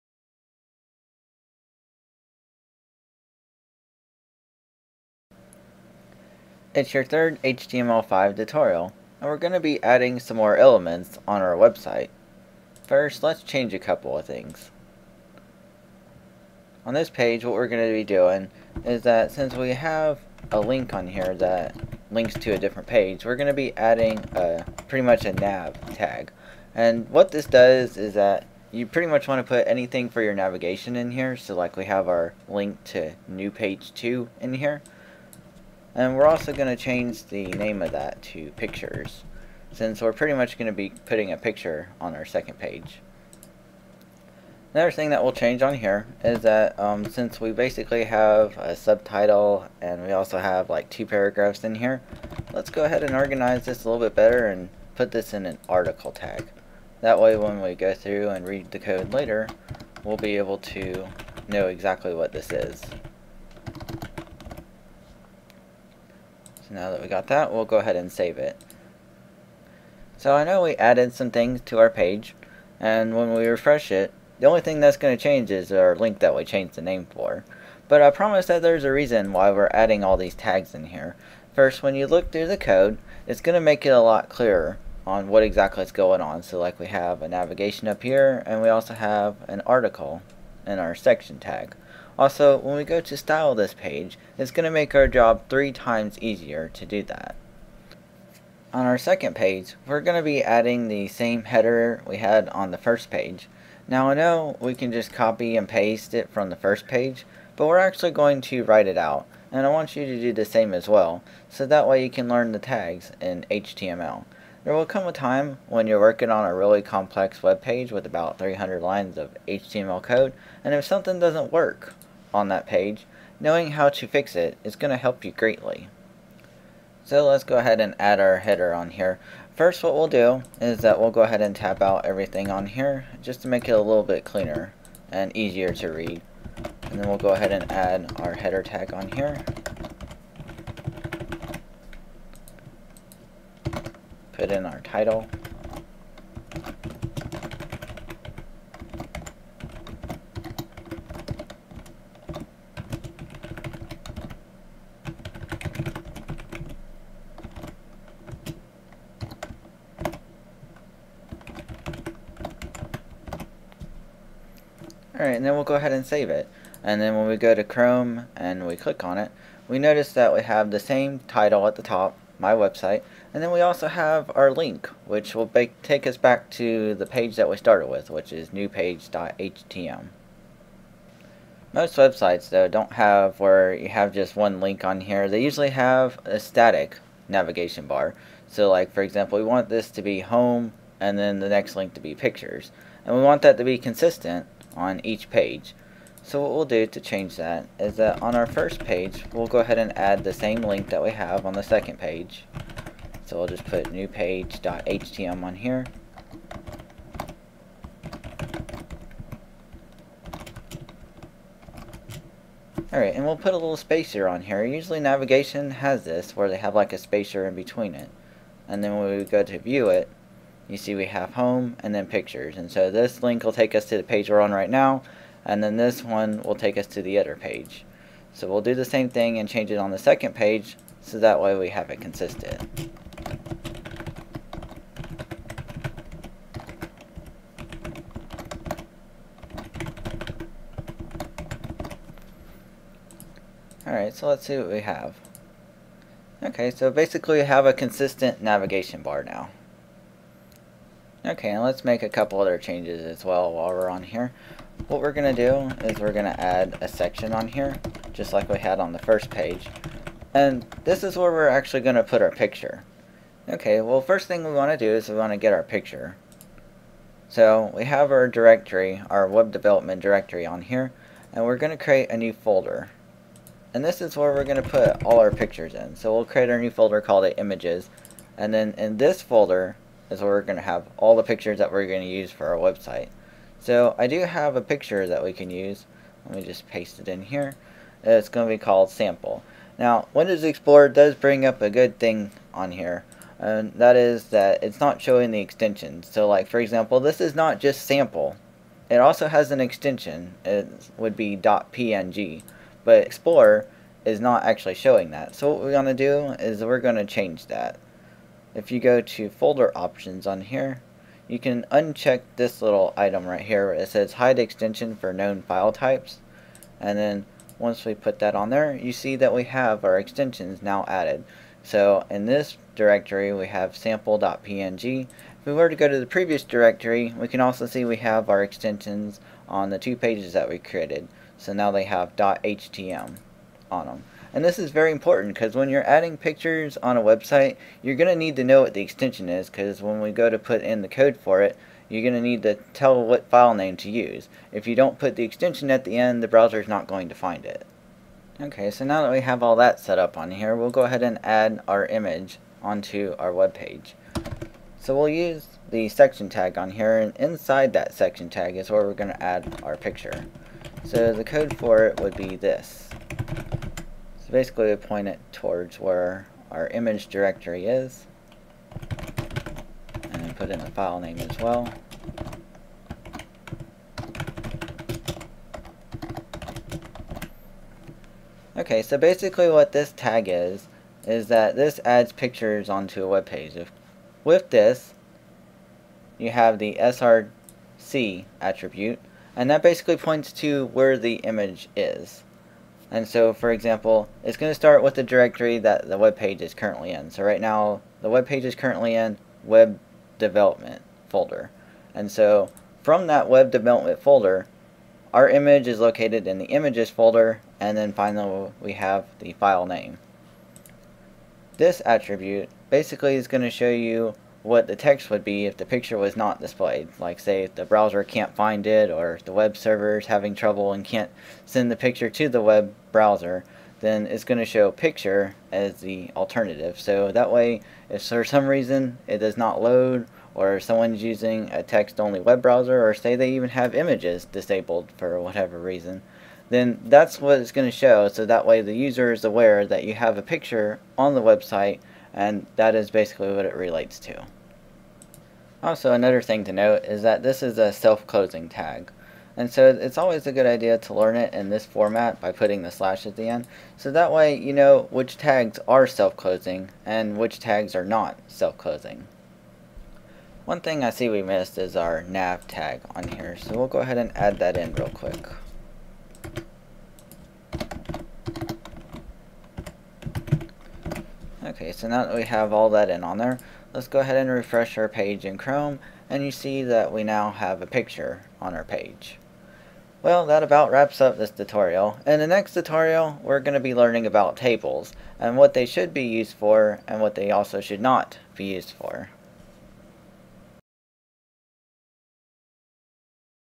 HTML5 / CSS3 Tutorial #3 (Adding an Image)
Inserting an Image onto our website to make it more prettyful (yes, prettyful)

All tutorials will work in the latest version of Chrome, Firefox, Opera, Safari, IE 9+

Note: IE 8 will not work.